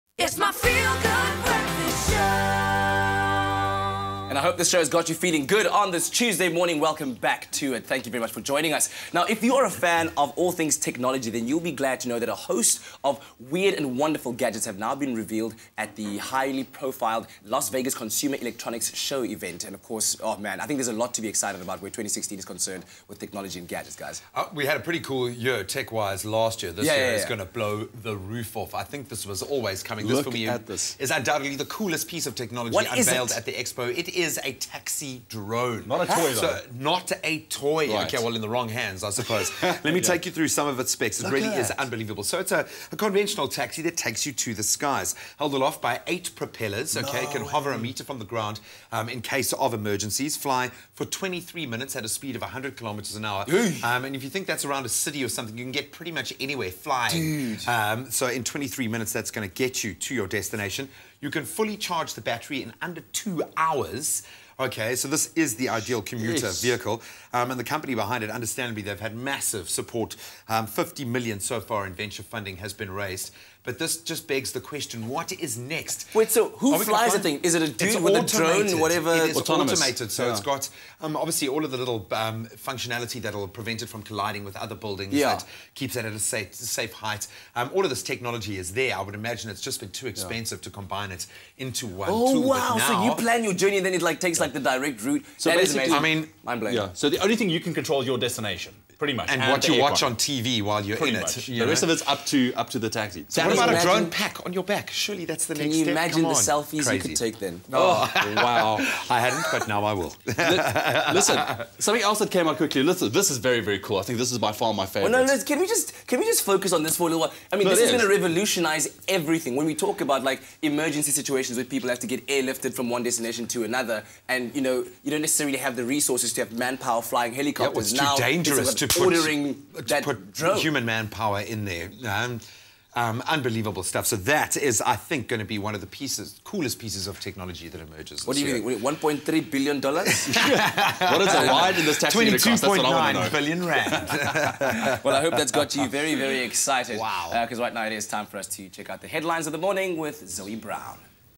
Highlights at CES 2016
If you’re a fan of technology, a host of weird and wonderful gadgets have just been unveiled at the high-profile Las Vegas Consumer Electronics Show event, and boy is there a lot to be excited about in 2016!